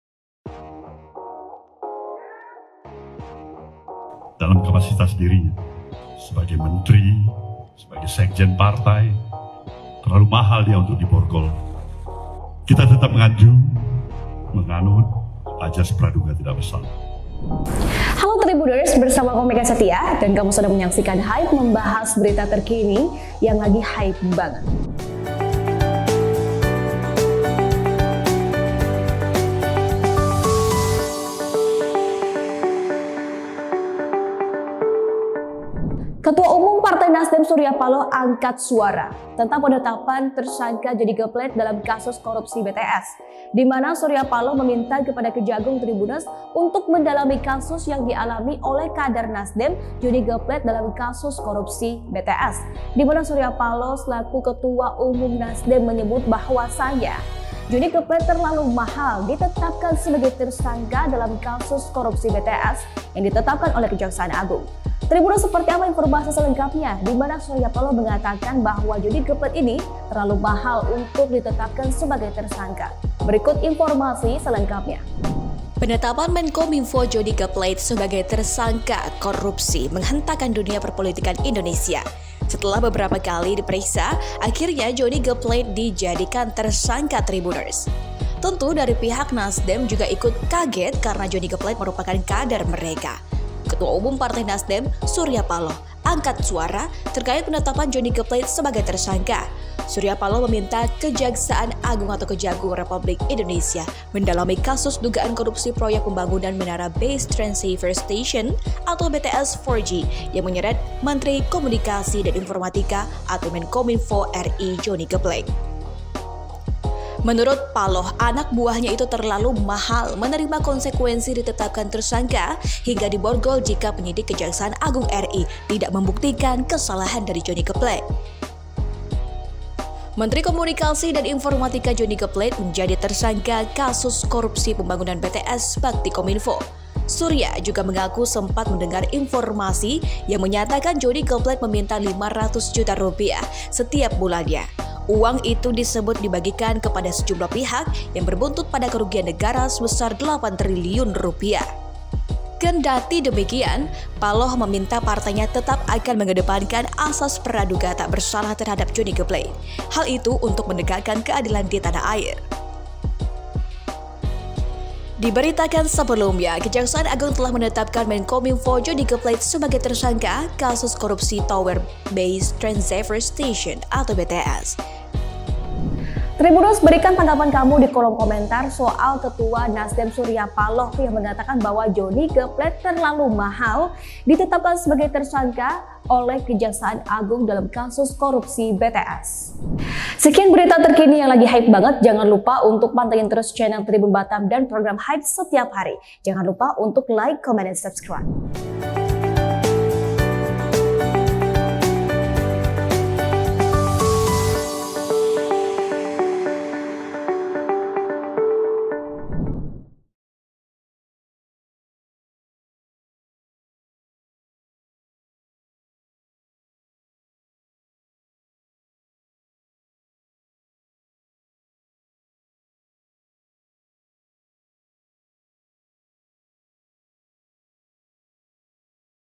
Surya Paloh Minta Kejagung Dalami Kasus Johnny G Plate, Sebut Anak Buahnya Terlalu Mahal
Ketua Umum Partai NasDem, Surya Paloh angkat suara terkait penetapan Johnny G Plate sebagai tersangka. Surya Paloh Minta Kejagung Dalami Kasus Johnny G Plate, Sebut Anak Buahnya Terlalu Mahal.

Surya Paloh meminta Kejaksaan Agung (Kejagung) RI mendalami kasus dugaan korupsi proyek pembangunan menara base transceiver station (BTS) 4G yang menyeret Menteri Komunikasi dan Informatika (Menkominfo) RI Johnny G Plate.

"Kalau tidak ada pendalaman lebih untuk mengumpulkan bukti-bukti yang lebih memberatkan.

Ya semakin lebih sedih lagi kita terlalu mahal dia untuk di borgol. Dalam kapasitas dirinya sebagai menteri, sebagai sekjen partai terlalu mahal, terlalu mahal," kata Surya dalam konferensi persnya di NasDem Tower, Jakarta, Rabu (17/5/2023).

Host : Megah
EV : Bregas